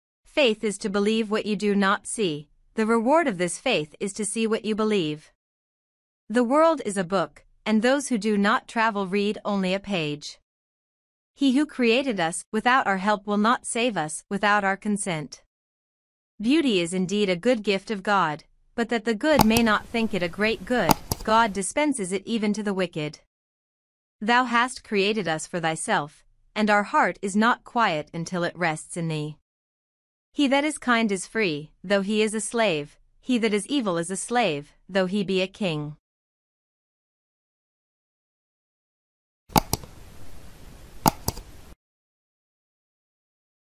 Saint Augustine Quotes.
Saint Augustine

Saint Augustine was born in 354AD in the city of Thagaste on the African continent. He was the son of a pagan bishop. At the age of sixteen, Augustine became a Christian and began studying at the Monastery of Saint Pius. In 383AD, Augustine was ordained a priest and later became the Bishop of Hippo in 399AD.

Augustine became a renowned theologian and philosopher. He is best known for his book The City of God, in which he argued that the City of God was a perfect society that could be found on earth. Augustine also wrote The Confessions, which is considered one of the greatest works of Christian literature.